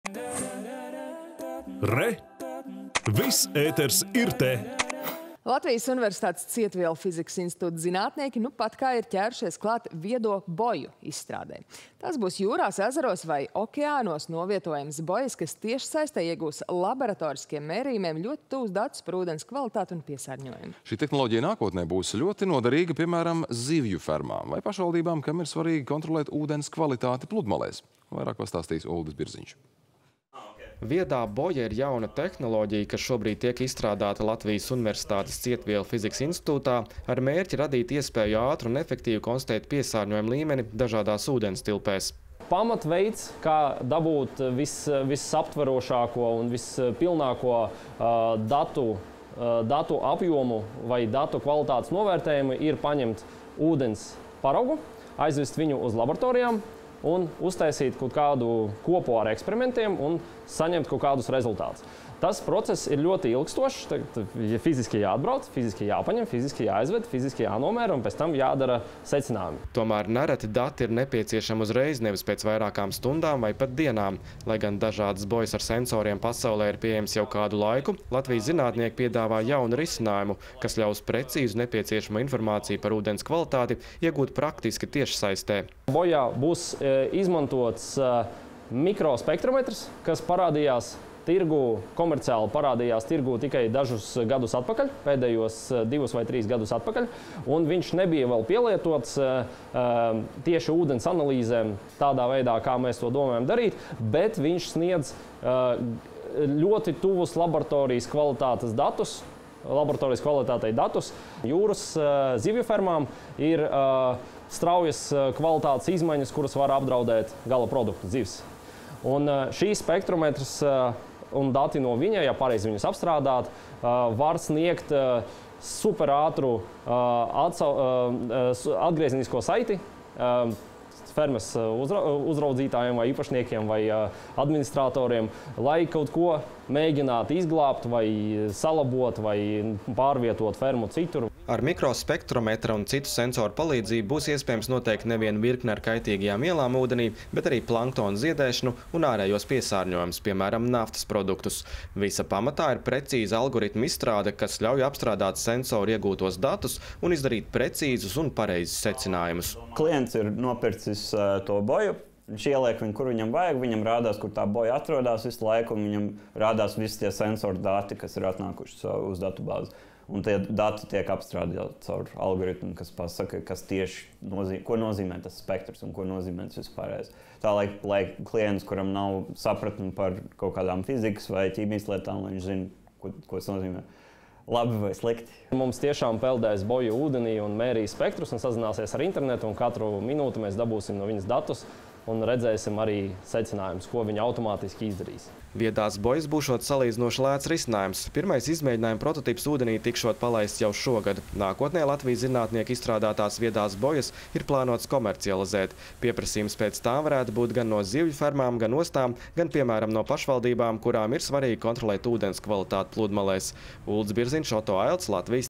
Latvijā top viedās bojas ūdens kvalitātes mērīšanai
Latvijas universitātes Cietvielu fizikas institūta zinātnieki nupat kā ķērušies klāt viedo boju izstrādei. Tās būs jūrās, ezeros vai okeānos novietojamas bojas, kas tiešsaistē iegūs laboratoriskiem mērījumiem ļoti tuvus datus par ūdens kvalitāti un piesārņojumu. Šī tehnoloģija nākotnē būs ļoti noderīga, piemēram, zivju fermām, vai pašvaldībām, kurām ir svarīgi kontrolēt ūdens kvalitāti pludmalēs. Plašāk tūdaļ pastāstīs kolēģis Uldis Birziņš